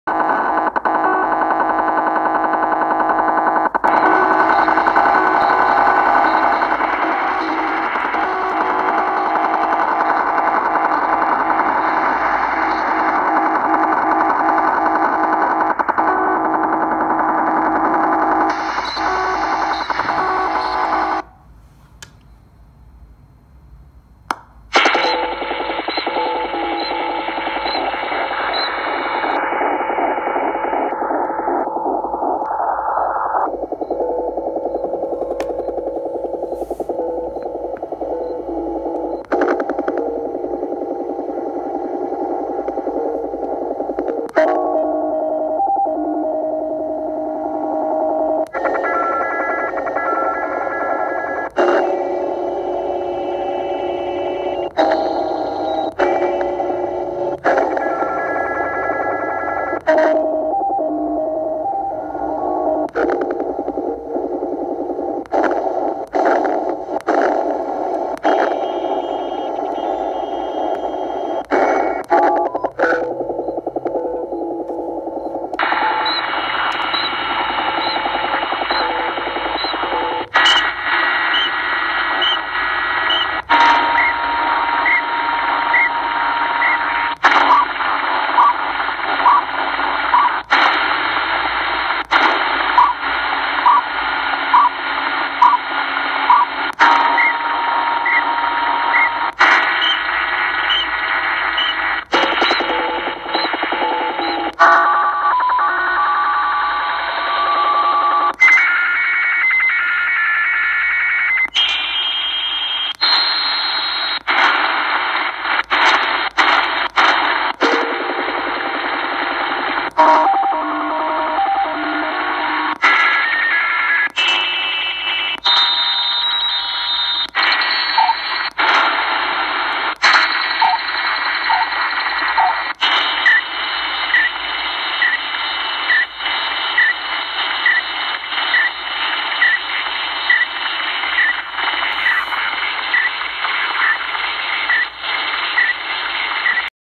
Slot Machine 🎰 4154kHz / Ichihara 190km
This signal has existed since I first heard shortwave radio when I was in elementary school. It sounds strange when I listen in SSB mode.
QTH:TOKYO JAPAN